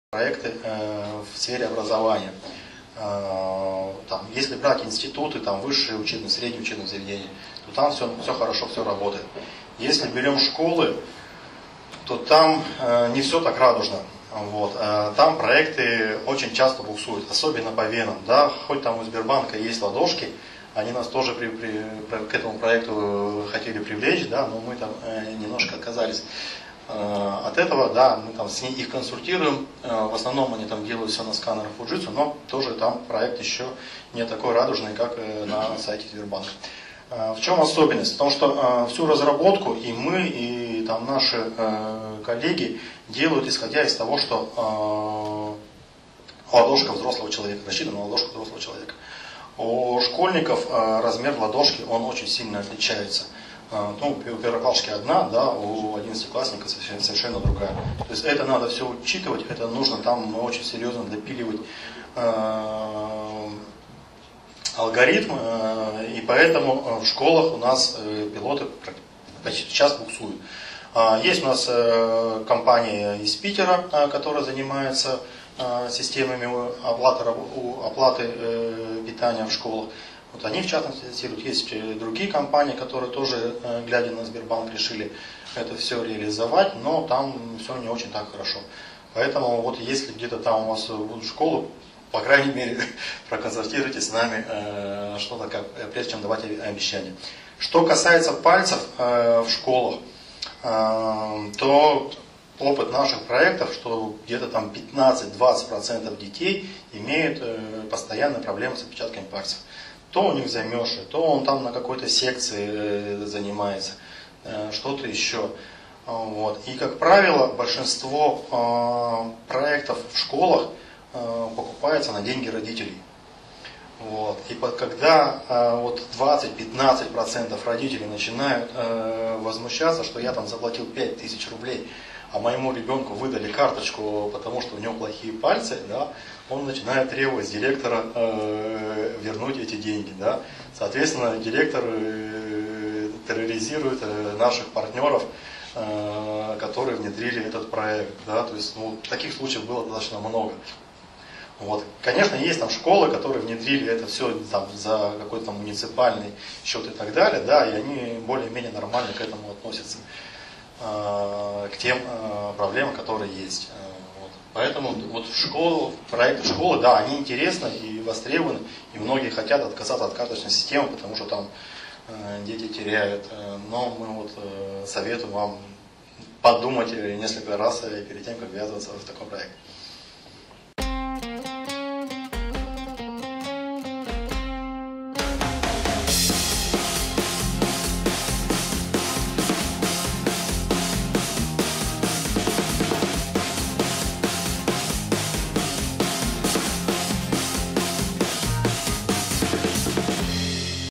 1 сентября. Проблемы биометрии в школах
1 сентября. Проблемы биометрии в школах
В.Ю.Богачев, "Прософт-Биометрикс"
Проекты в сфере образования. Если брать институты, высшие учебные, средние учебные заведения, то там все хорошо, все работает. Если берем школы, то там не все так радужно. Вот. Там проекты очень часто буксуют, особенно, по венам.
Хоть там у Сбербанка есть "Ладошки", они нас тоже к этому проекту хотели привлечь, да, но мы там немножко отказались от этого. Да, мы там их консультируем, в основном они там делают все на сканерах Fujitsu, но тоже там проект еще не такой радужный, как на сайте Сбербанка.
В чем особенность? В том, что всю разработку и мы, и там наши коллеги делают, исходя из того, что ладошка взрослого человека, рассчитано на ладошку взрослого человека. У школьников размер ладошки – он очень сильно отличается: ну, у первоклашки одна, да, а у одиннадцатиклассника совершенно другая. То есть это надо все учитывать, это нужно там очень серьезно допиливать алгоритмы, и поэтому в школах у нас пилоты подчас буксуют.
Есть у нас компания из Питера, которая занимается системами оплаты питания в школах. Вот они, в частности, есть и другие компании, которые тоже, глядя на Сбербанк, решили это все реализовать, но там все не очень так хорошо. Поэтому вот если где-то там у вас в школах, по крайней мере проконсультируйтесь с нами, что да как, прежде, чем давать обещания.
Что касается пальцев в школах, то опыт наших проектов, где-то там 15… 20% детей имеют постоянно проблемы с отпечатками пальцев. То у них замерзшие, то он там на какой-то секции занимается, что-то еще. Вот, и, как правило, большинство проектов в школах покупается на деньги родителей. Вот и когда 20… 15% родителей начинает возмущаться, что я там заплатил 5 000 рублей, а моему ребенку выдали карточку, потому что у него плохие пальцы, он начинает требовать с директора вернуть эти деньги, да. Соответственно, директор терроризирует наших партнеров, которые внедрили этот проект. Да, то есть, ну, таких случаев было достаточно много.
Вот. Конечно, есть там школы, которые внедрили это все за какой-то там муниципальный счет и так далее, да, и они более-менее нормально к этому относятся, к тем проблемам, которые есть.
Поэтому вот в школах, проекты в школах – да, они интересны и востребованы, и многие хотят отказаться от карточной системы, потому что там дети теряют, но мы вот советуем вам подумать несколько раз перед тем, как ввязываться в такой проект.